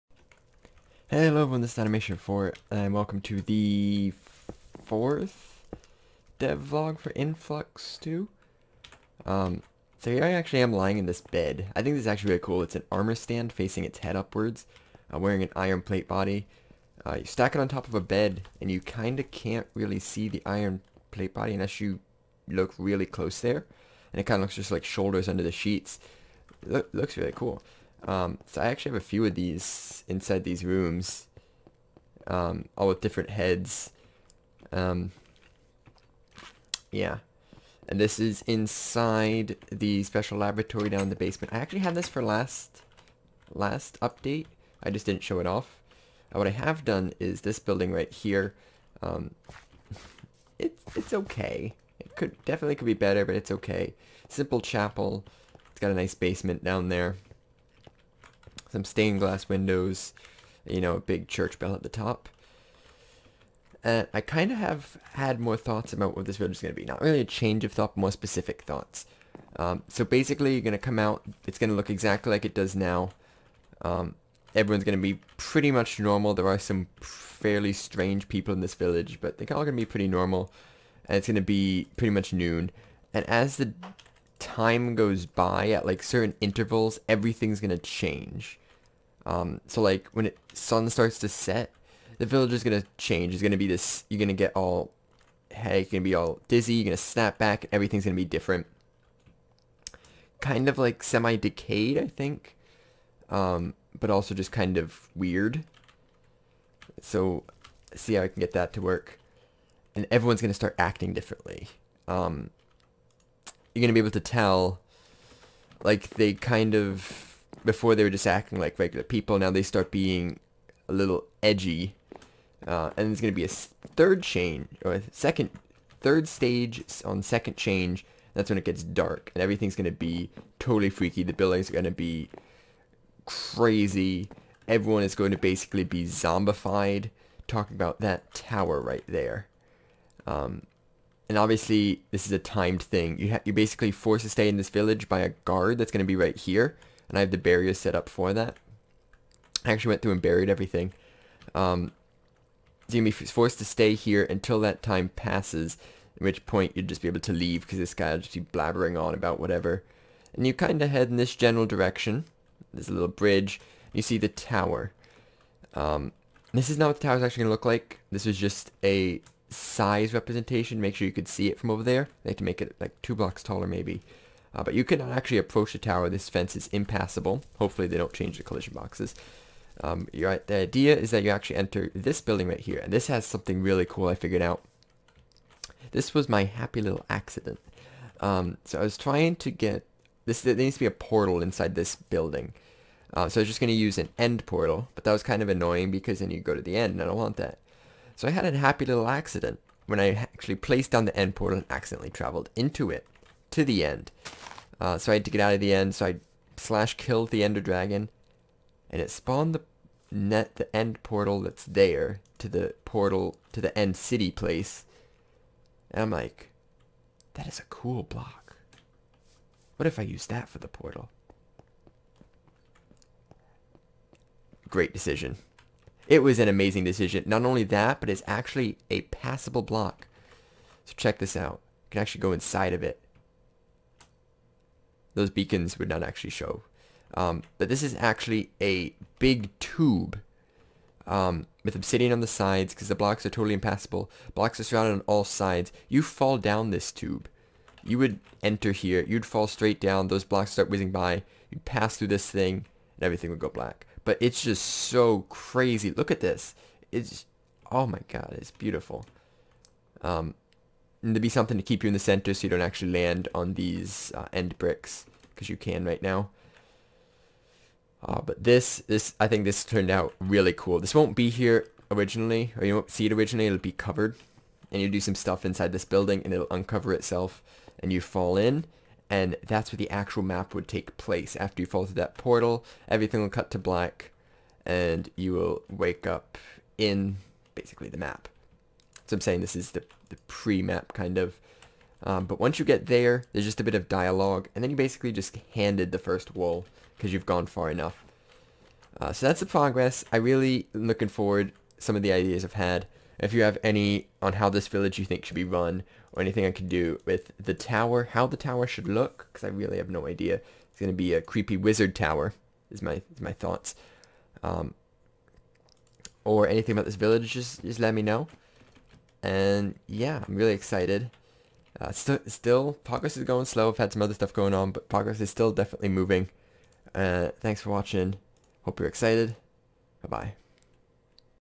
Influx 2 Dev Vlog 5 - A Happy Little Accident
[I forgot what goes here] This is a map, and the progress i'm making on it.